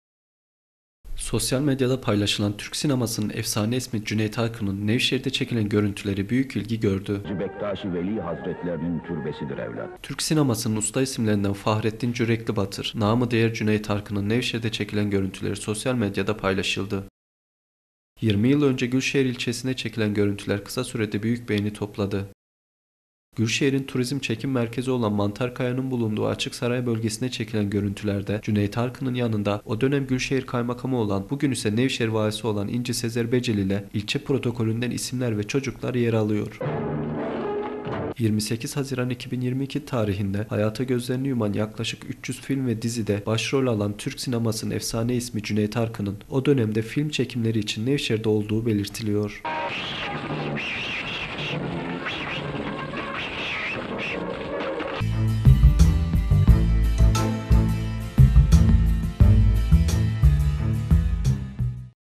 NEVŞEHİR - BU GÖRÜNTÜDEKİLERİ TANIDINIZ MI?
Sosyal medyada paylaşılan Türk sinemasının efsane ismi Cüneyt Arkın’ın Nevşehir’de çekilen görüntüleri büyük ilgi gördü.